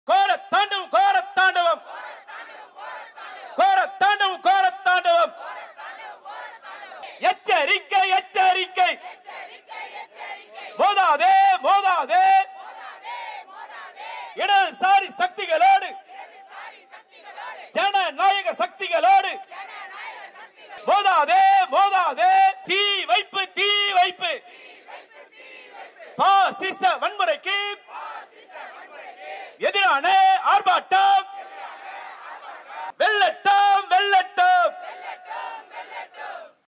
திரிபுராவில் பாஜகவின் பாசிச வன்முறையை எதிர்த்து ஆர்ப்பாட்டம்!
திரிபுராவில் பாஜகவின் பாசிச வன்முறையை முறியடித்து ஜனநாயகத்தை மீட்போம்!
சென்னை மாவட்டக் குழுக்கள் ஒருங்கிணைக்கும் அனைத்துக்கட்சி பெருந்திரள் ஆர்ப்பாட்டம் !